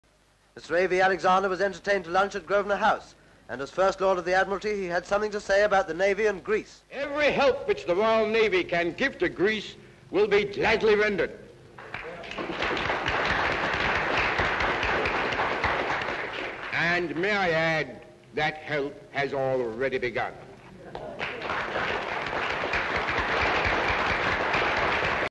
First Lord of the Admiralty talks about support for Greece (1940)
Albert Victor Alexander, 1st Earl Alexander of Hillsborough (First Lord of the Admiralty) speaks about his support for Greece

Full Description:

ENGLAND: London: Westminster: Mayfair: Grosvenor House:
EXT

First Lord of the Admiralty, Royal Navy, Greece
Background: Albert Victor Alexander, 1st Earl Alexander of Hillsborough (First Lord of the Admiralty) speaks about his support for Greece